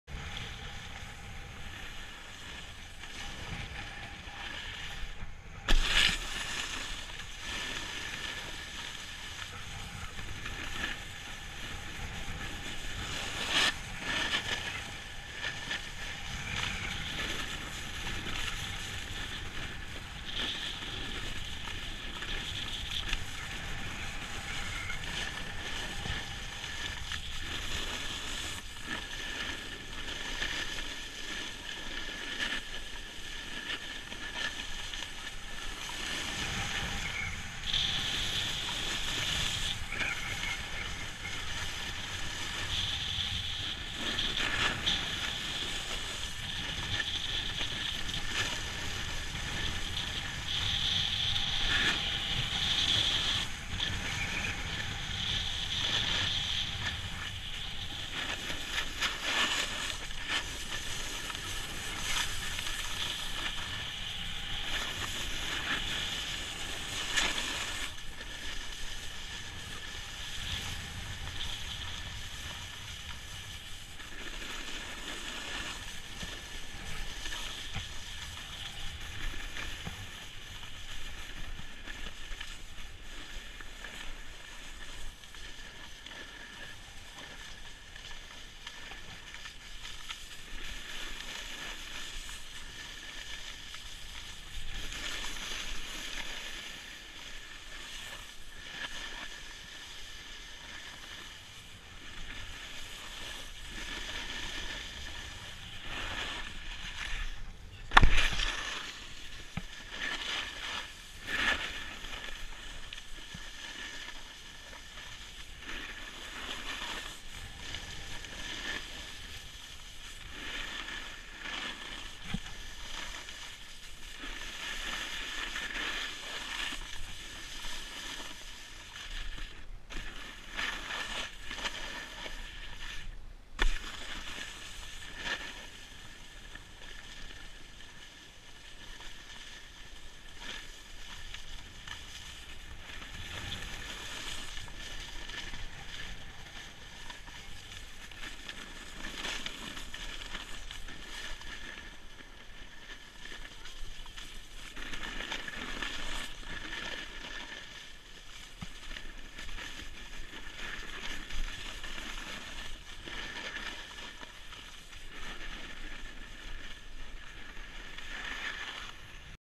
Trace: Skiing - Brian Calderwood at Mountain High
Trace auto edited Brian Calderwood's skiing session at Mountain High on Dec 11, 2015.

Runs: 6
Min of Skiing: 18
Vertical M: 5.8k ft
Distance: 5.3 mi
Calories Burned: 329
Max Speed: 34.1 mph
Max sustained speed: 26.9